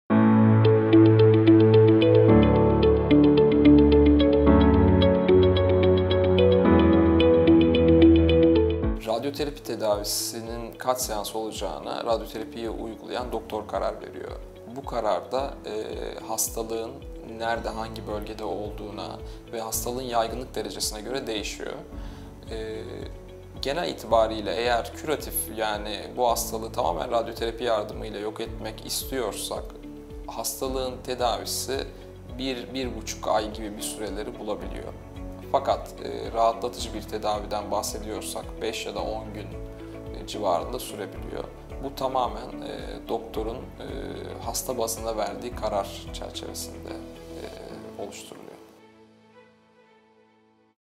Radyoterapi Tedavisi Kaç Seans Uygulanır?
Acıbadem Bodrum Hastanesi Radyasyon Onkolojisi Uzmanı Dr. İlker Tosun  "Radyoterapi" üzerine soruları yanıtlıyor.